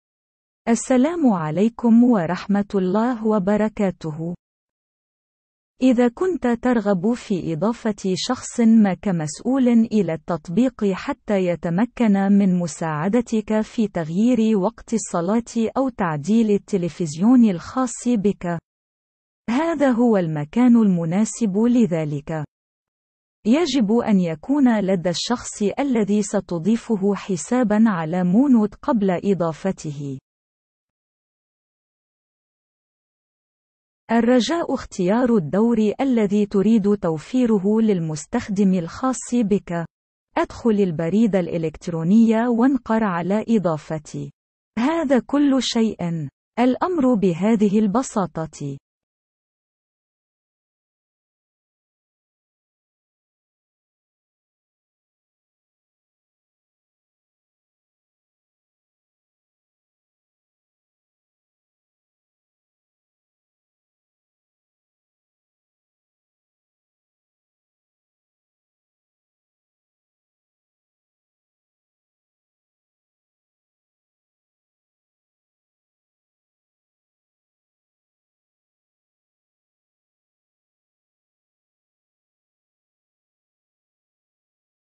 كيفية تغيير الدور على Moonode
1 - إذا كنت ترغب في إضافة شخص ما كمسؤول للتطبيق حتى يمكنه مساعدتك في تغيير مواقيت الصلاة أو تعديل التليفزيون الخاص بك. هذا هو المكان المناسب لذلك.

2 - يجب أن يكون لدى الشخص الذي ستضيفه حسابًا على Moonode قبل إضافته.

3 - الرجاء اختيار الدور الذي تريد توفيره للمستخدم الخاص بك. أدخل البريد الإلكتروني وانقر على إضافة. هذا كل شيء. الأمر بهذه البساطة.

4 - هذا المثال هو شخص تمت إضافته كمسؤول ثانوي لتعديل مواقيت الصلاة ، عند الاتصال ، سيكون لدى هذا الشخص قسم الصلاة كما هو موضح هنا.